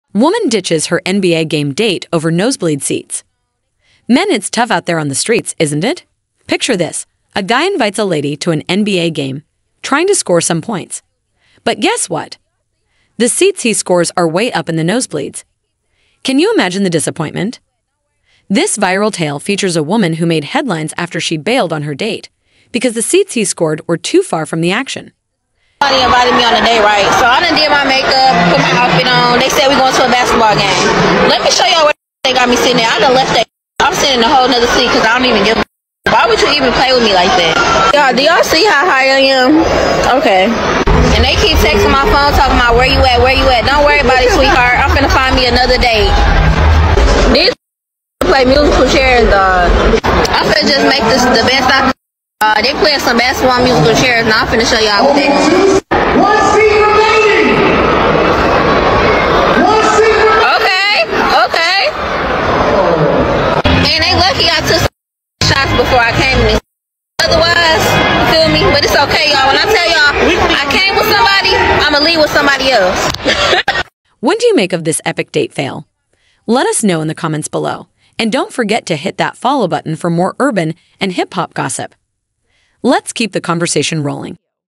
Woman Ditches Her NBAGame Date Over Nosebleed Seats
Men, it’s tough out there on the streets, isn’t it? Picture this